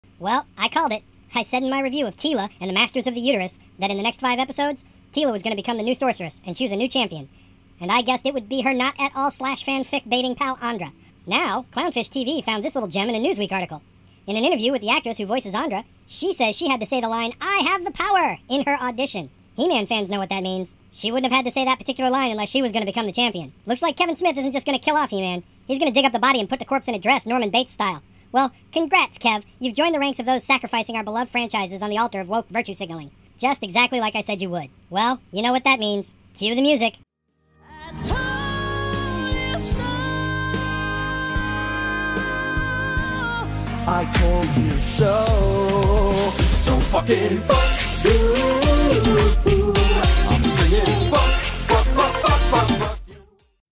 Master Of The Universe Revelation - Andra Replacing He-Man - Netflix | Dark Nook
Is Andra the new He Man?  Looks like Kevin Smith lied again.  In Netflix's Masters of the Universe Revelation He Man dies and is replaced by Andra.  Kevin Smith's woke He Man is a disaster... and I told you so.

I hope you enjoy this Dark Nook Video.

💀 𝐖𝐄𝐋𝐂𝐎𝐌𝐄 𝐓𝐇𝐄 𝐃𝐀𝐑𝐊 𝐍𝐎𝐎𝐊 💀